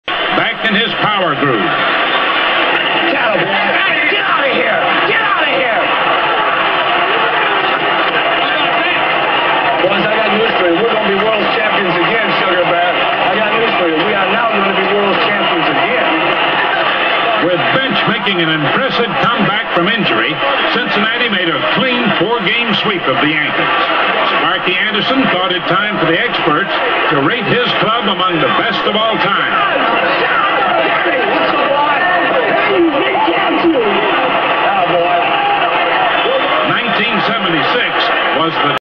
Cincinnati Red's Manager Sparky Anderson Screaming As Johnny Bench Homers!
Cincinnati Reds Managers was excited and fired-up as Johnny Bench homered in the 1976 World Series at Yankee Stadium, and shared his thoughts with his team as they celebrated in the dugout!